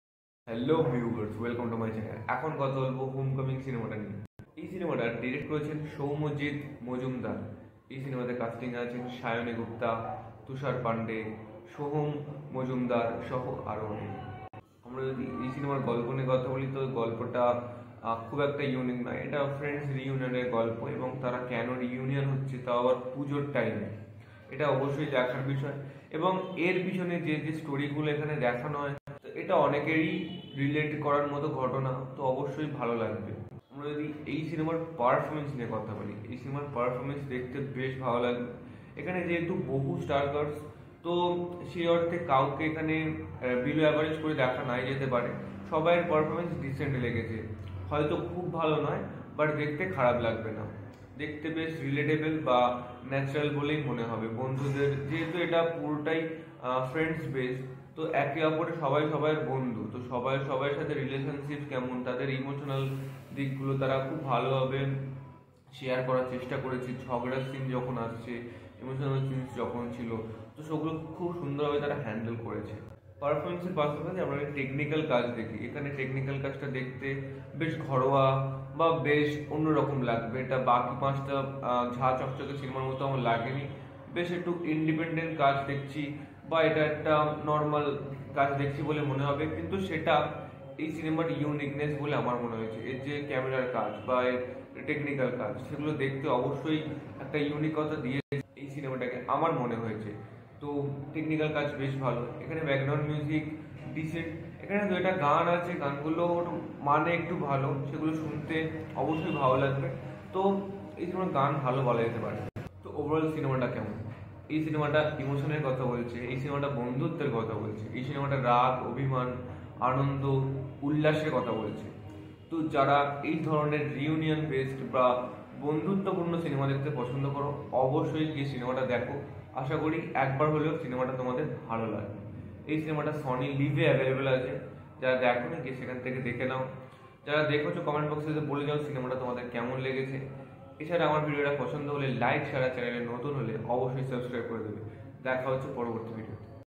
HOMECOMING | MOVIE REVIEW | SAYANI G | TUSHAR P | SOHAM M | TUHINA D | SOUMYAJIT M | SONY LIV
Reviewed by Souvik Modak